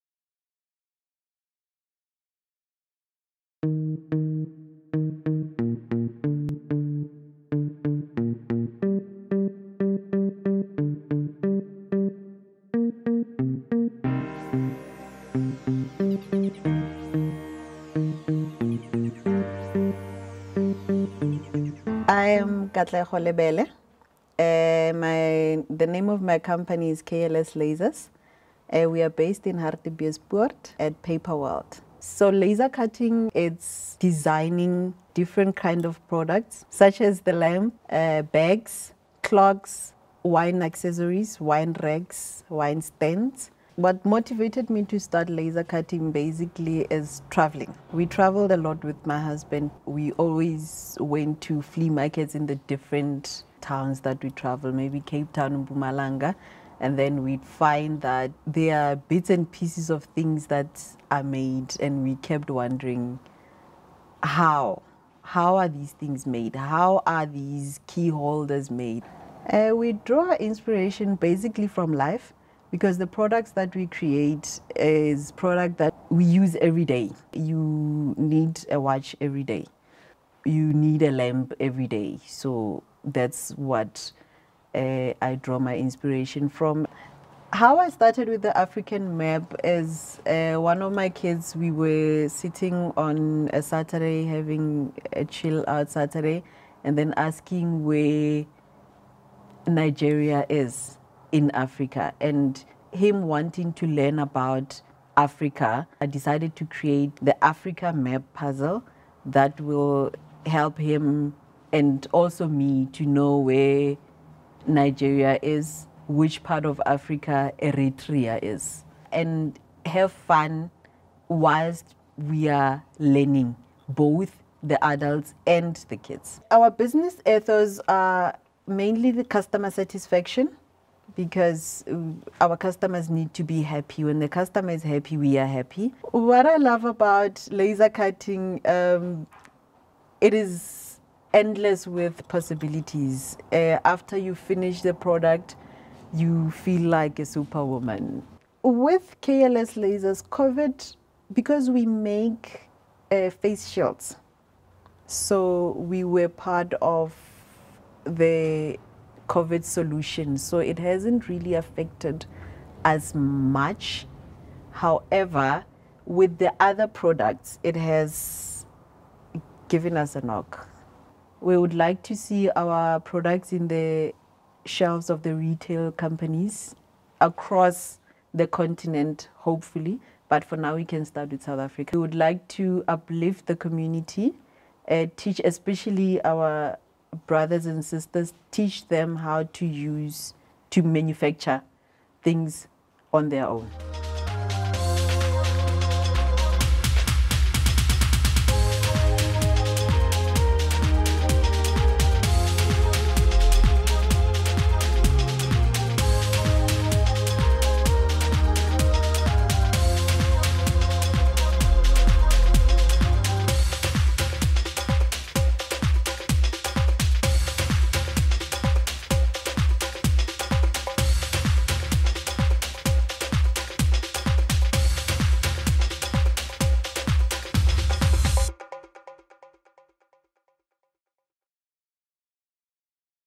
KLS Lasers: Katlego Lebele - Hartbeespoort (Paperworld), North West, South Africa
We recently went to Hartbeesport to visit KLS lasers. They are a laser cutting and engraving company which mainly specialise in laser cut home decor and personalised gifting. KLS lasers was established in 2018 by a couple from Rustenburg. Their passion for travel sparked and interest in how crafts were made, and when researching this they came across a laser cutting machine, which they later bought and started making simple items like earrings. They then taught themselves how to make bigger and more complexed designs. They have since employed 2 young adults from Lethabong, Rustenburg. KLS wishes to grow and transfer the skills to their own community, especially to empower the youth and enable them to create things of their own.